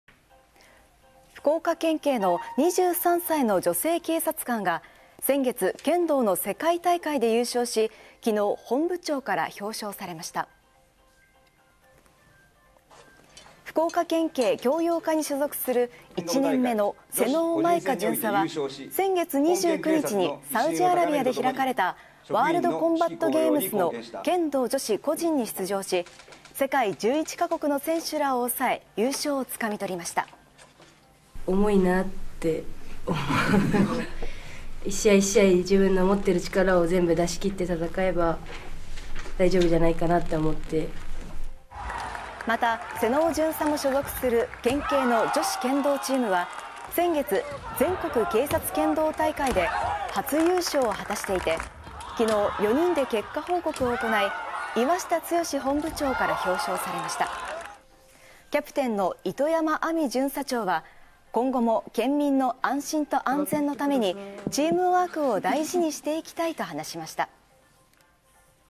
福岡県警の女性巡査（２３） 剣道で世界一に！
福岡県警の２３歳の女性警察官が先月、剣道の世界大会で優勝し、１３日、本部長から表彰されました。

福岡県警教養課に所属する１年目の妹尾舞香巡査（２３）は、先月２９日にサウジアラビアで開かれた「ワールドコンバットゲームズ」の剣道女子個人に出場し、世界１１カ国の選手らを抑え、優勝をつかみ取りました。

妹尾舞香巡査は、「（世界大会のメダルは）重いなぁって思いました。

１試合、１試合、自分の持っている力を全部出し切って闘えば大丈夫じゃないかなと思って闘いました」と振り返りました。

また、妹尾巡査も所属する県警の女子剣道チームは先月、全国警察剣道大会で初優勝を果たしていて、１３日、４人で結果報告を行い、岩下剛本部長から表彰されました。

キャプテンの糸山亜美巡査長（２８）は、「今後も県民の安心と安全のためにチームワークを大事にしていきたい」と話しました。